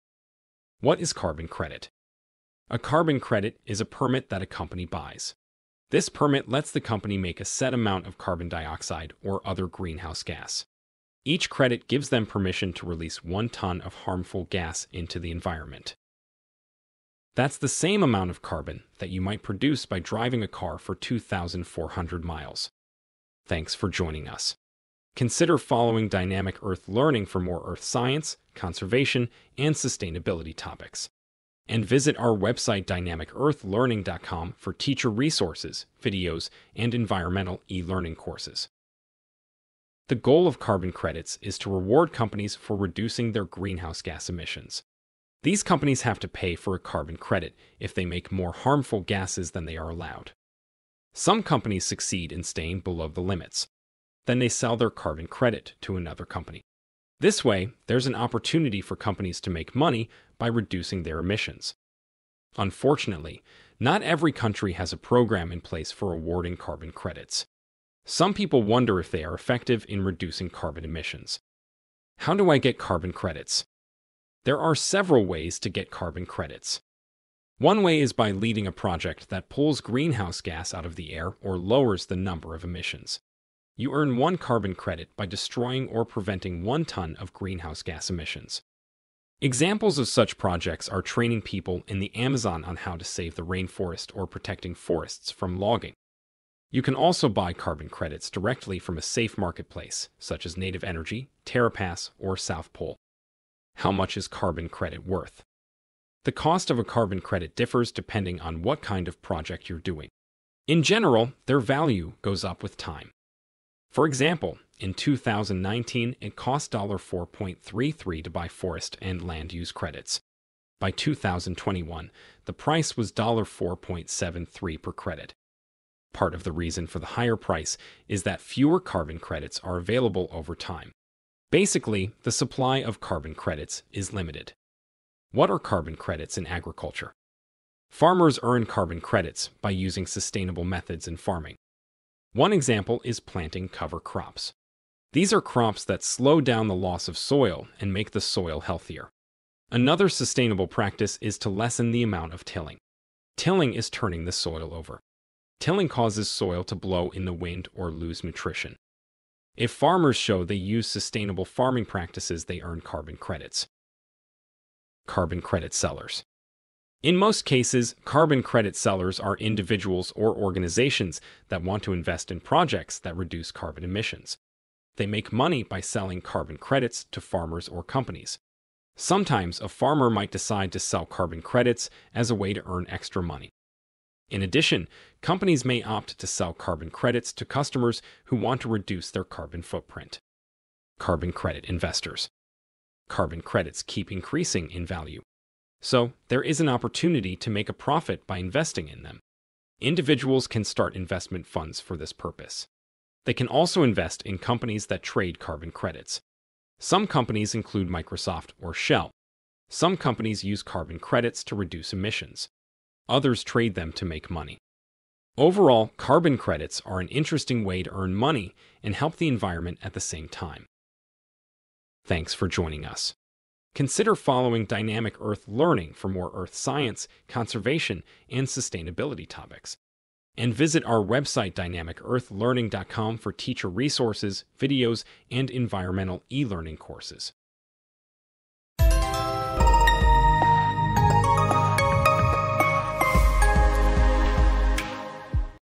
What is Carbon Credit? How Do I Get Carbon Credit? Sustainability
What is Carbon Credit? How much is carbon credit worth? These are important questions. Other topics we cover include; what is carbon credits in agriculture, carbon credit sellers, carbon credit investors, and how do i get carbon credits?

🏫 Check out topics for earth science on Dynamic Earth Learning's Teachers Pay Teachers!